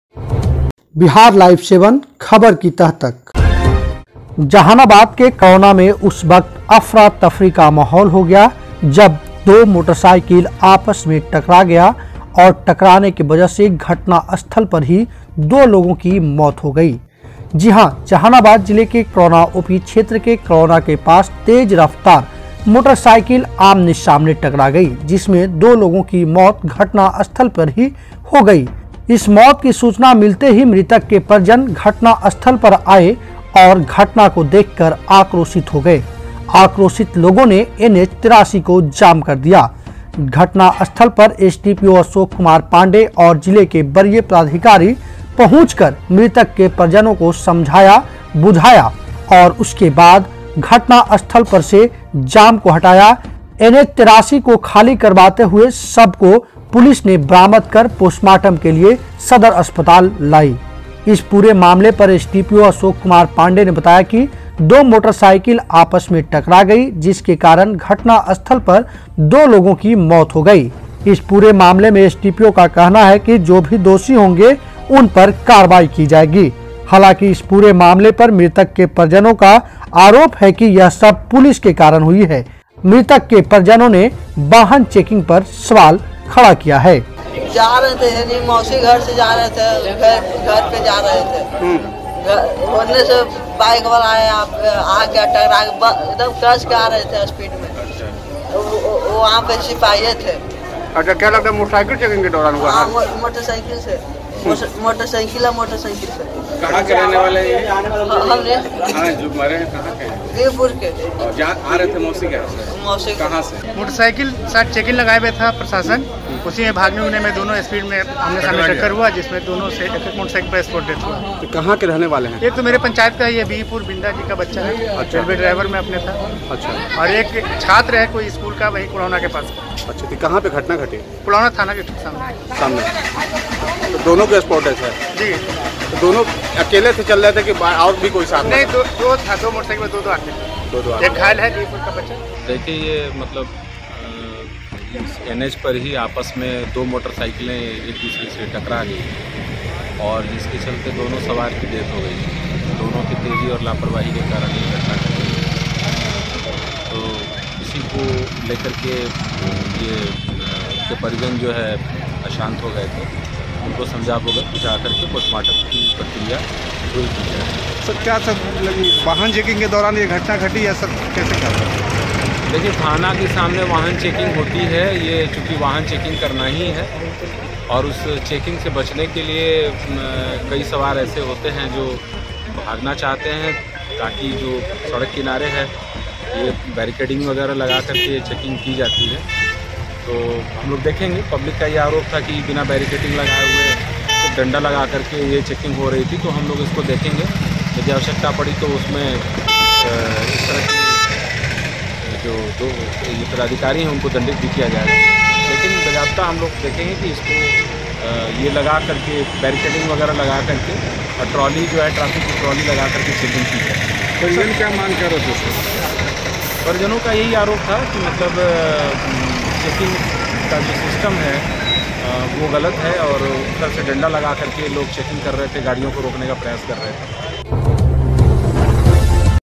Jehanabad के कडौना में दो तेज रफ्तार मोटरसाइकिल आपस में आमने-सामने टकराई,दो की मौत,NH83 को किया जाम
Jehanabad के कडौना में दो तेज रफ्तार मोटरसाइकिल आपस में आमने-सामने टकराई,दो की मौत,NH83 को किया